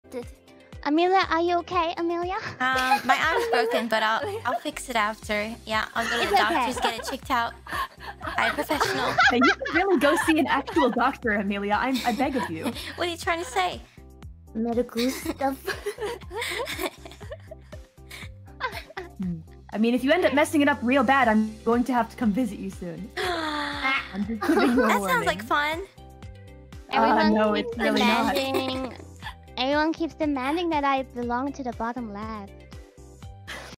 Amelia's Broken Arm Pt.2
Short Clip of Amelia and her broken arm from the HoloLiveEN Collab Stream